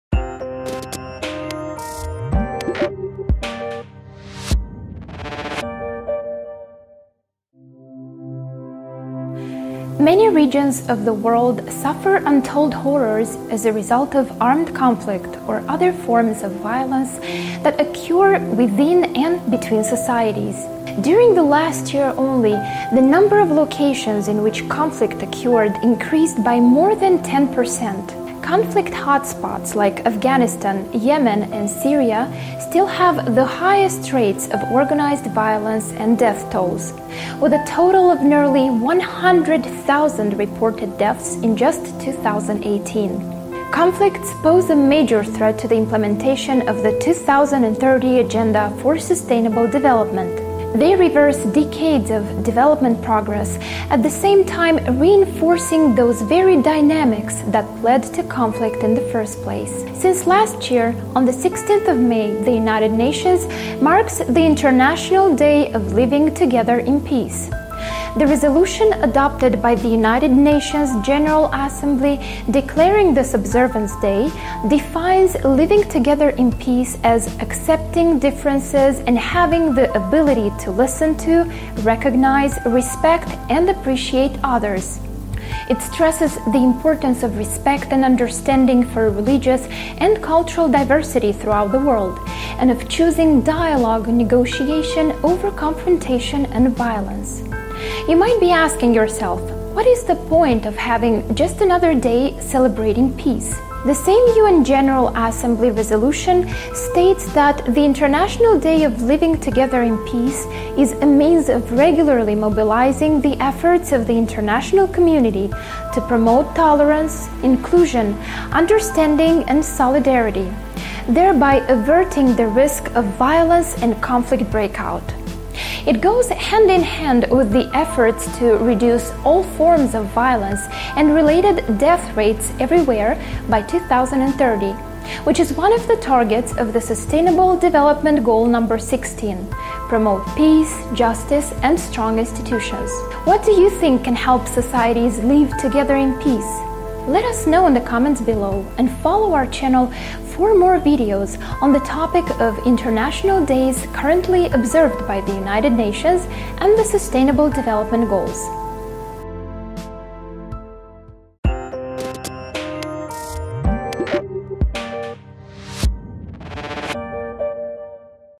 International Day of Living Together in Peace
Since 2018, on the 16th of May, the United Nations marks the International Day of Living Together in Peace. But why do we need one more day designated to celebrate peace?